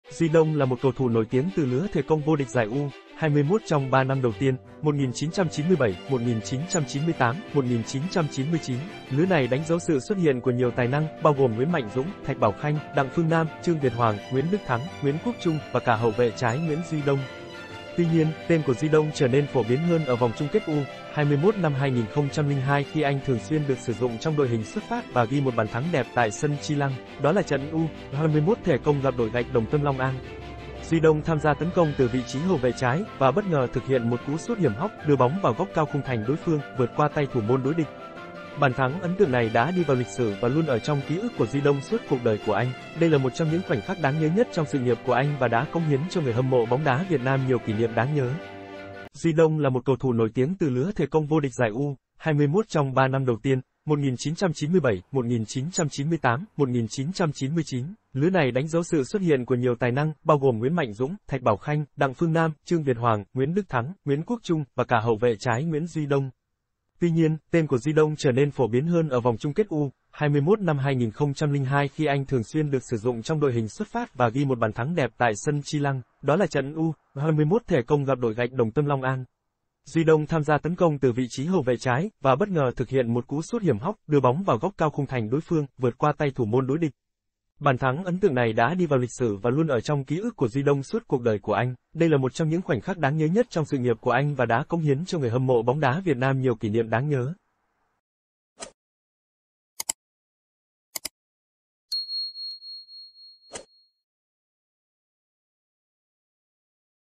KỶ NIỆM BÀN THẮNG ĐỂ ĐỜI CỦA NGUYỄN DUY ĐÔNG TRONG GIẢI U.21
Duy Đông là một cầu thủ nổi tiếng từ lứa Thể Công vô địch giải U.21 trong 3 năm đầu tiên (1997, 1998, 1999). Lứa này đánh dấu sự xuất hiện của nhiều tài năng, bao gồm Nguyễn Mạnh Dũng, Thạch Bảo Khanh, Đặng Phương Nam, Trương Việt Hoàng, Nguyễn Đức Thắng, Nguyễn Quốc Trung, và cả hậu vệ trái Nguyễn Duy Đông.